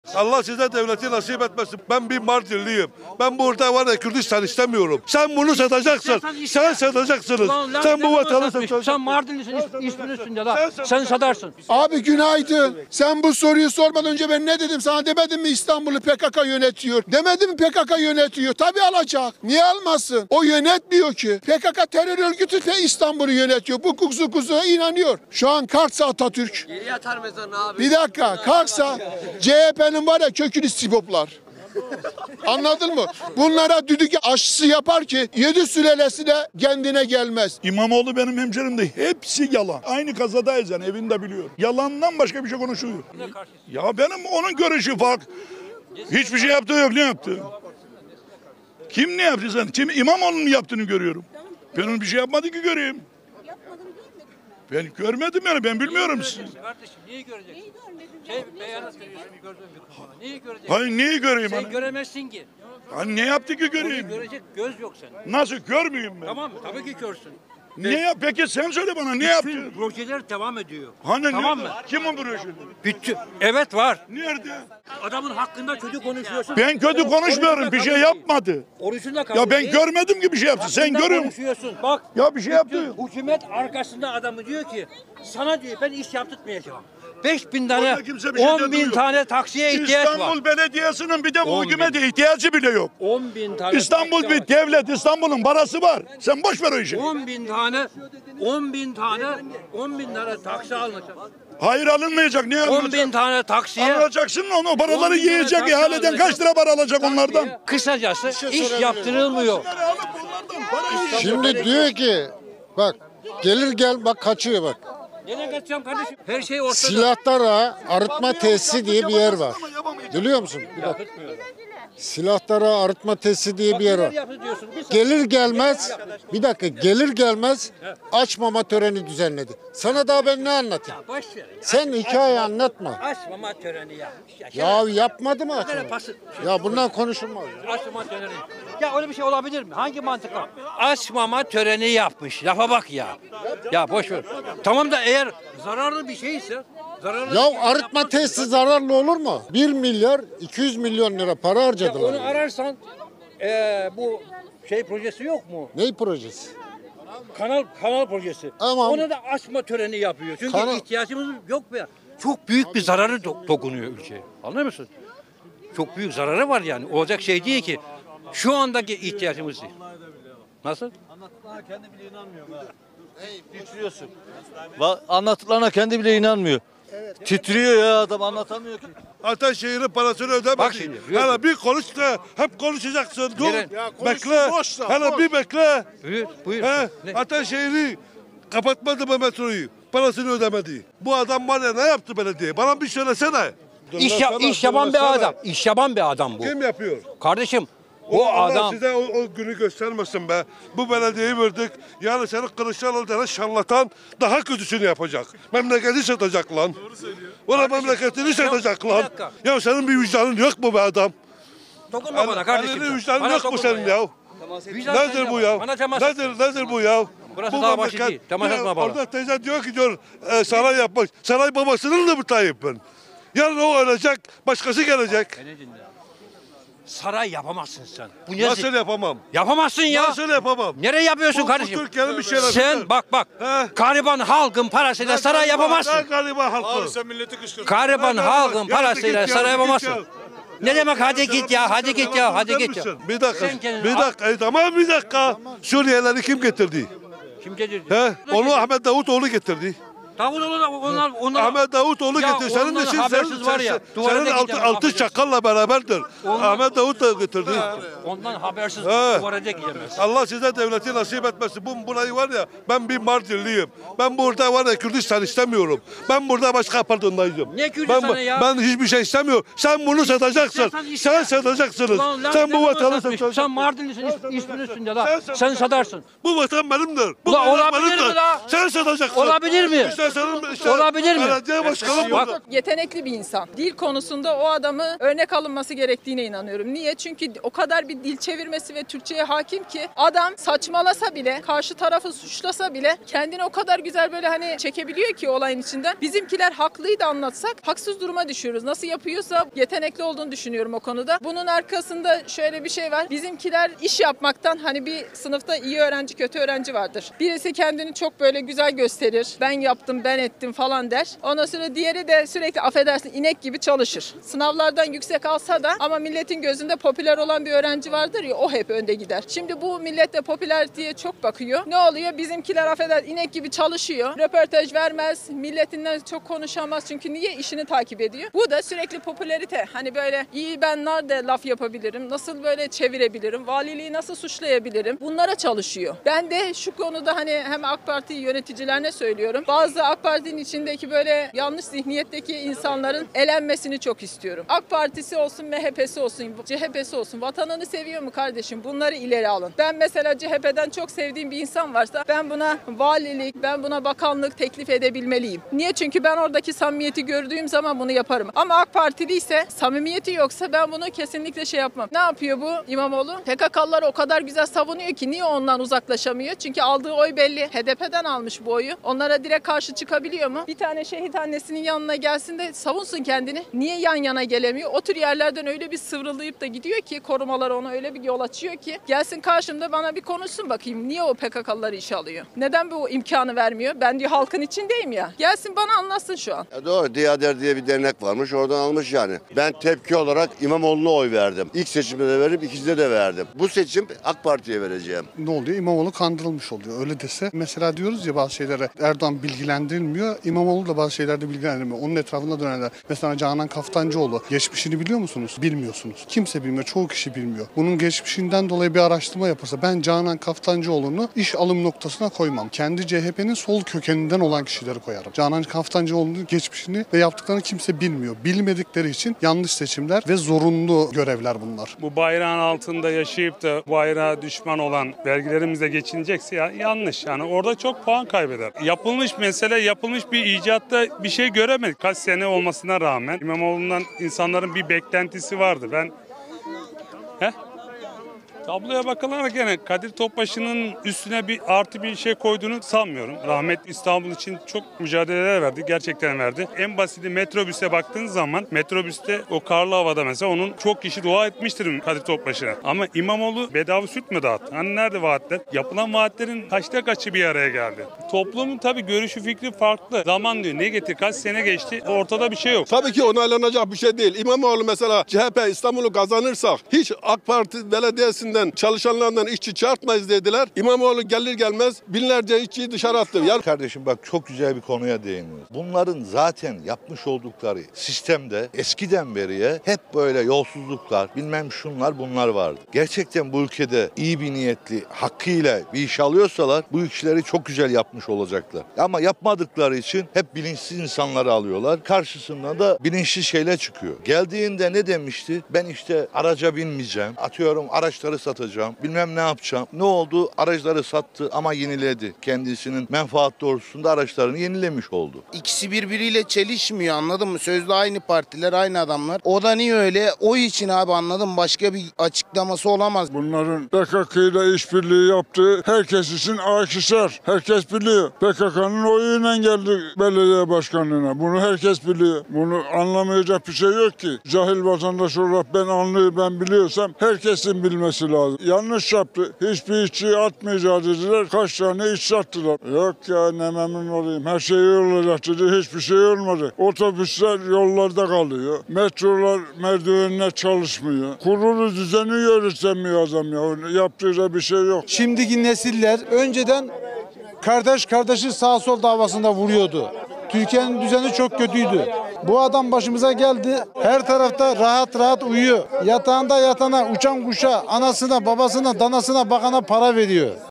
Mardinli Kürt İmamoğlu'nu Savunan Adamı Yerin Dibine Soktu..
VATANSEVER KÜRT CHP'LİYİ YERİN DİBİNE SOKTU